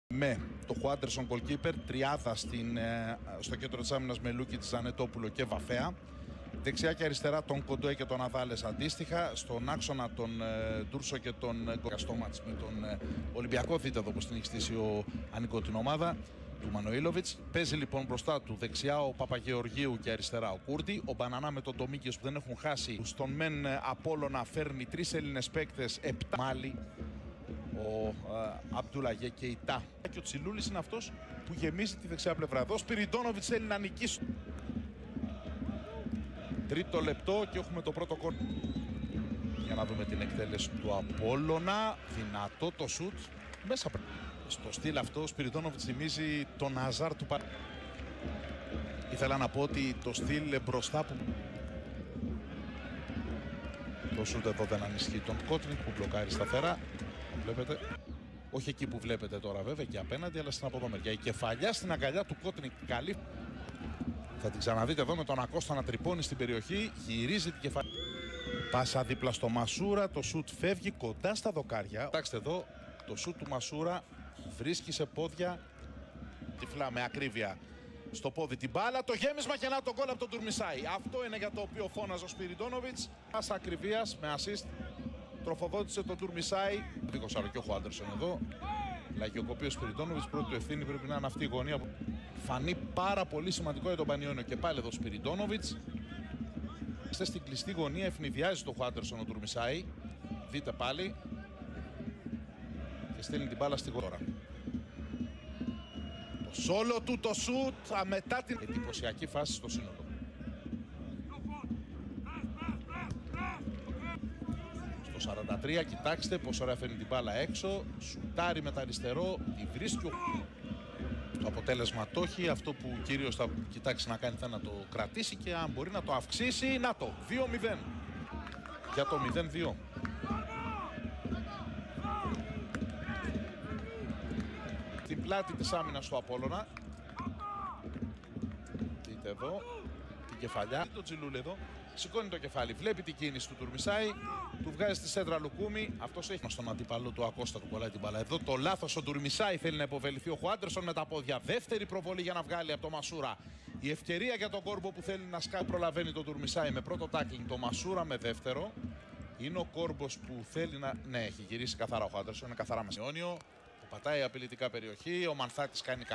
Aπόλλων Σμύρνης- Πανιώνιος 0-2| Στιγμιότυπα 5η Αγ. Superleague (30-9-2018)
Apollon-Panionios 0-2
Γήπεδο: "Γιώργος Καμάρας", Ριζούπολη

Διαιτητής: Φώτιος Φωτιάδης

Βοηθοί: Ιωάννης Καραγκιζόπουλος, Νικόλαος Πάγκαλος

4ος Διαιτητής: Ευάγγελος Μανούχος

ΑΠΟΛΛΩΝ (Μοντεαγούδο): Χουάντερσον, Λούκιτς, Τζανετόπουλος (46' Τριάδης), Βαφέας, Ναδάλες, Γκονζάλες, Ντούρσο, Μπεντινέλι (64' Ελ-Χελβέ), Ακόστα (59' Σικαμπάλα), Κοντοές, Γκαρός.

ΠΑΝΙΩΝΙΟΣ (Ανιγκό): Κότνικ, Κούρντι, Παπαγεωργίου, Ντομίνγκες, Μπανανά, Κεϊτά, Κόρμπος, Σπιριντόνοβιτς (82' Οικονομίδης), Μασούρας (93' Σαραμαντάς), Ντουρμισάι, Τσιλούλης (66' Μανθάτης).

Γκολ: 37', 50' Ντουρμισάι

Κάρτες: 51' Τσιλούλης, 57' Ακόστα, 57' Παπαγεωργίου, 59' Κόρμπος, 73' Λούκιτς